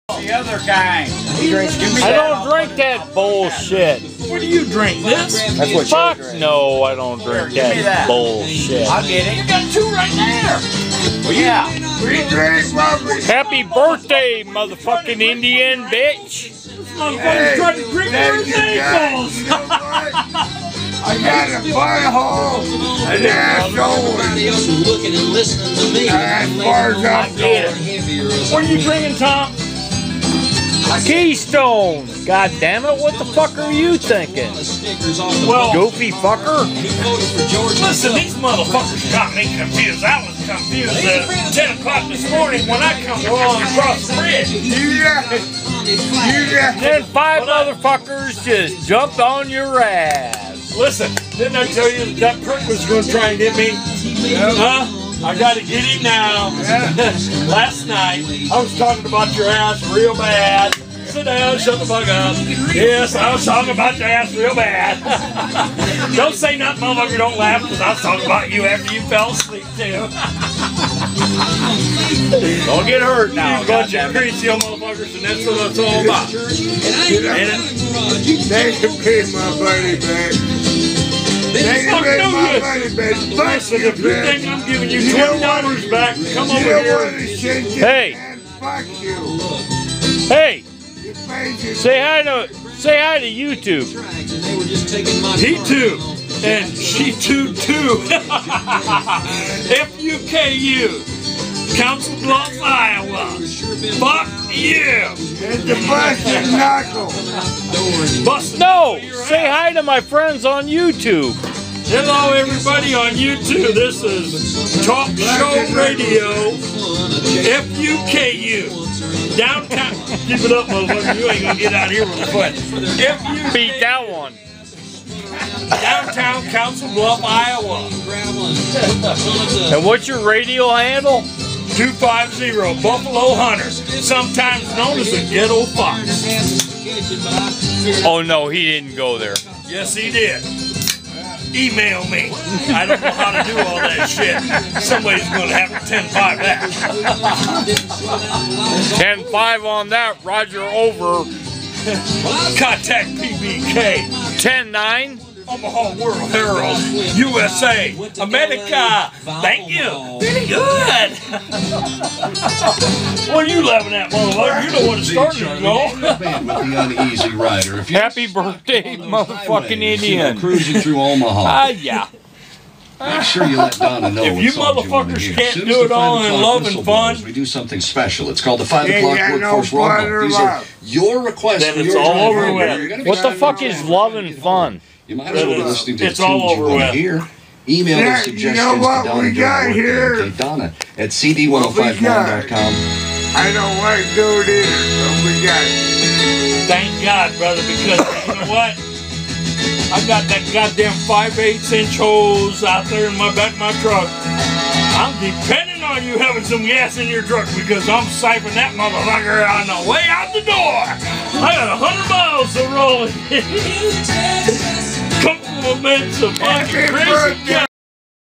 Injun Joe's Birthday Party Only from the Busted Knuckle Central!.MP4
Yep It was the Injuns birthday on 07Oct11, Come join the fun! It was great!!